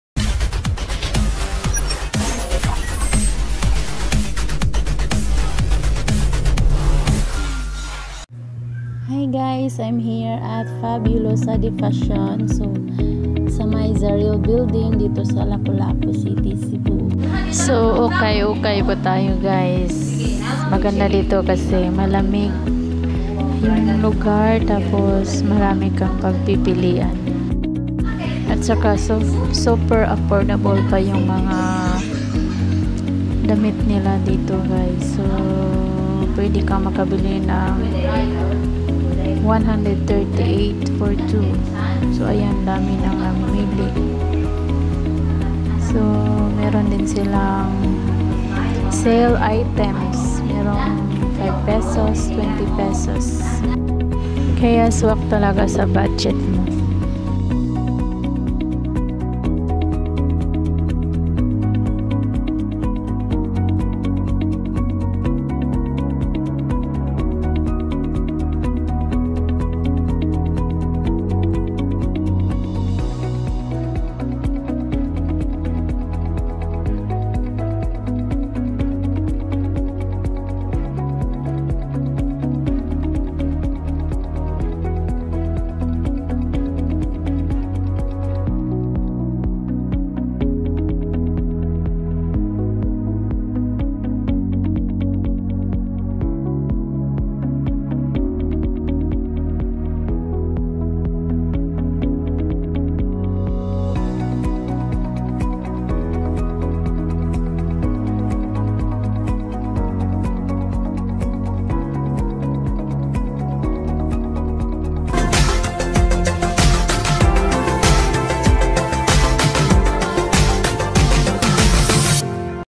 Ukay-Ukay for as low as P5 to P20/Fabulosa De Fashion @Lapu-Lapu City Cebu
Hello guys! This is Maria Nilda Mativo. In todays video I'm sharing you the best Ukay-Ukay store in Lapu-Lapu City...Hope you enjoy watching..